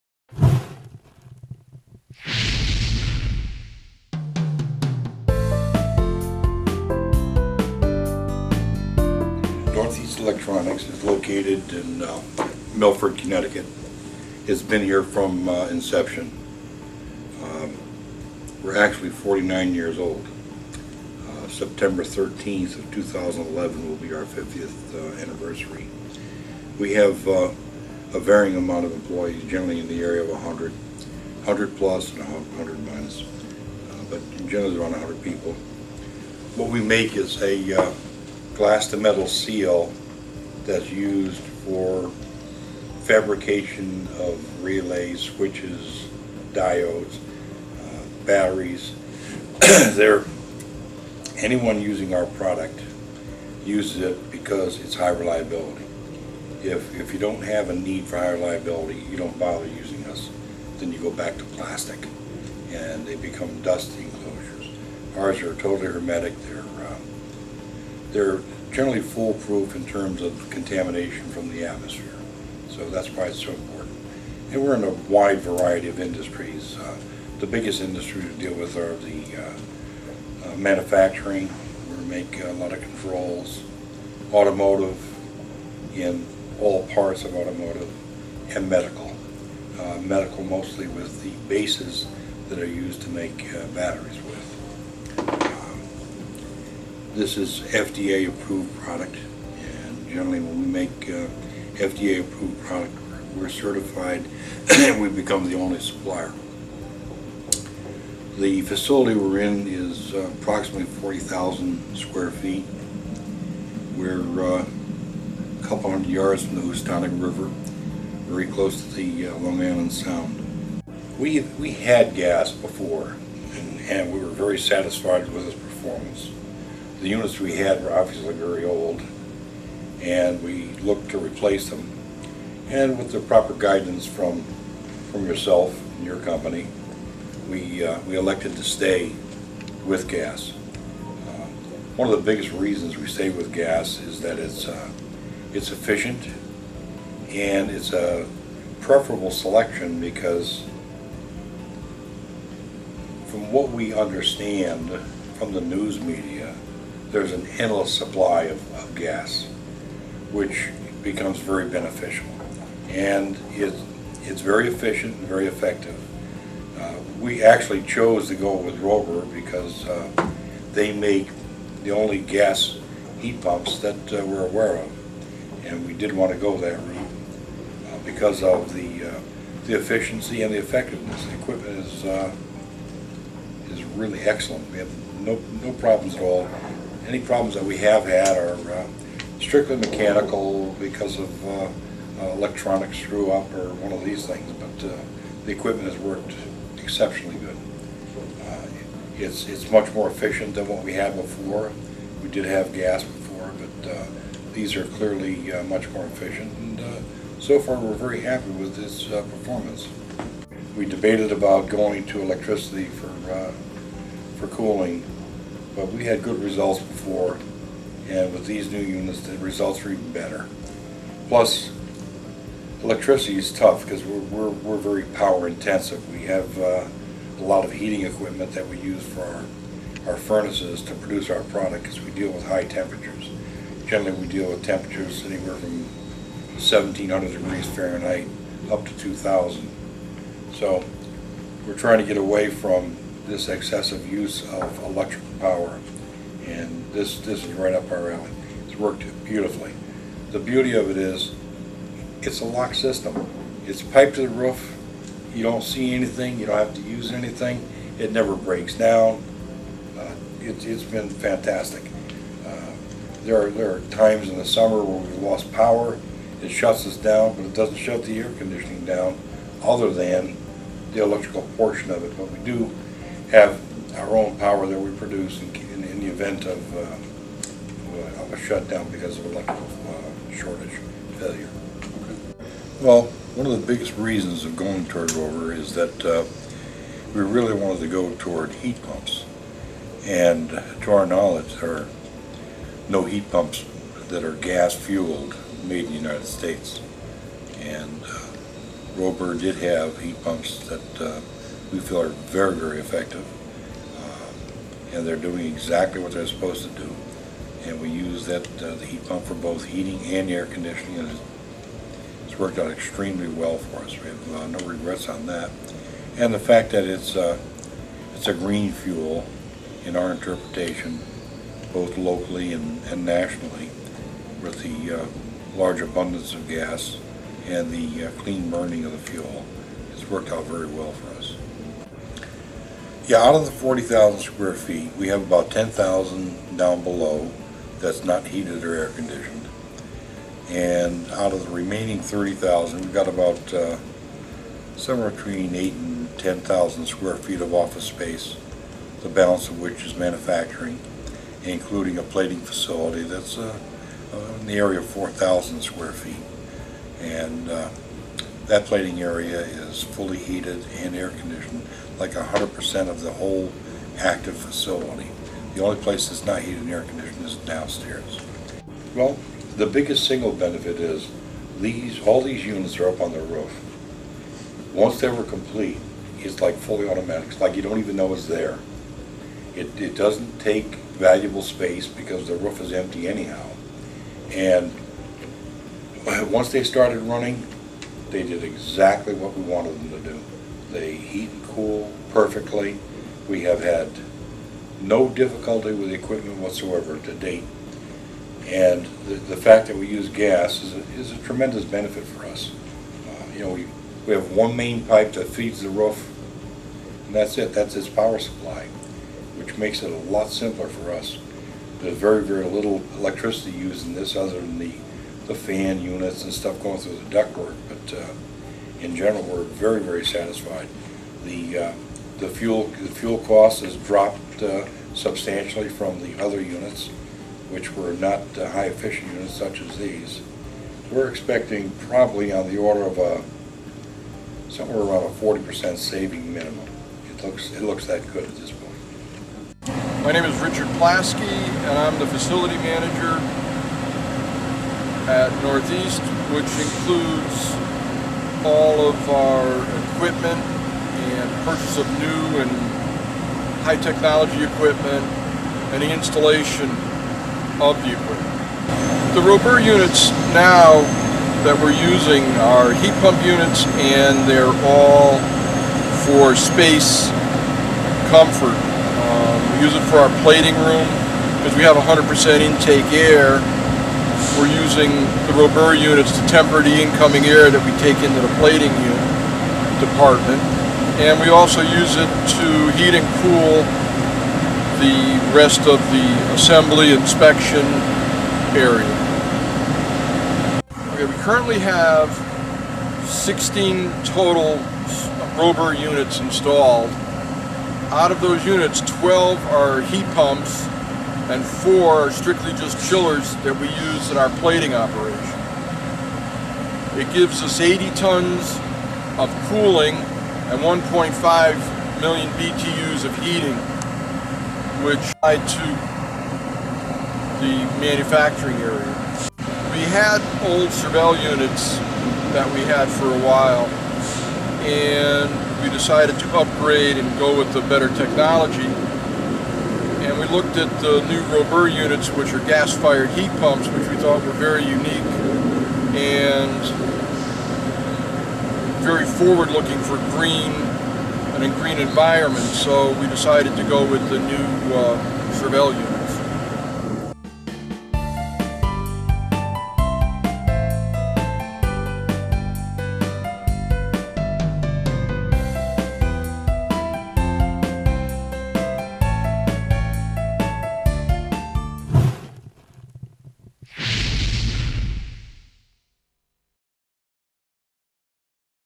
[English] ROBUR CASE STUDY: Northeast Electronics Corp
Heating capacity: 1,500,000 BTU/h
Cooling capacity: 80 TON

12 Gas Absorption Reversible Heat Pumps
4 Gas Absorption Chillers for process applications ACF TK